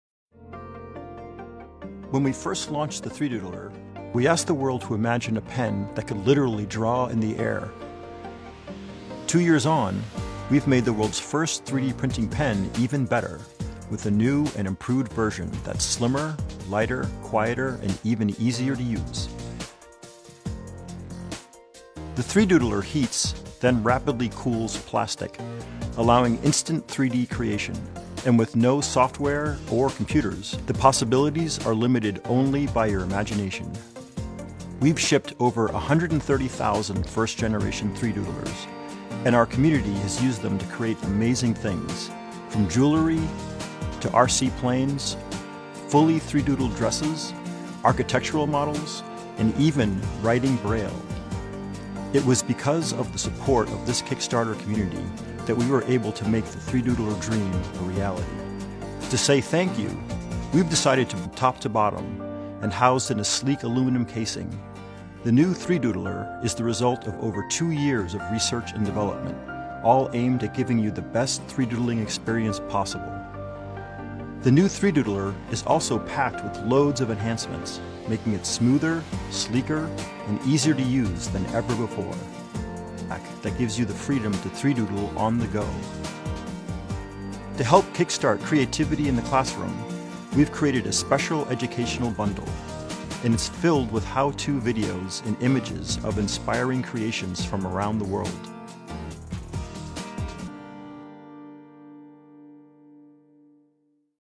3D Pen - Caneta que Desenha em 3 Dimensões
Vídeo que demonstra uma caneta que desenha em três dimensões.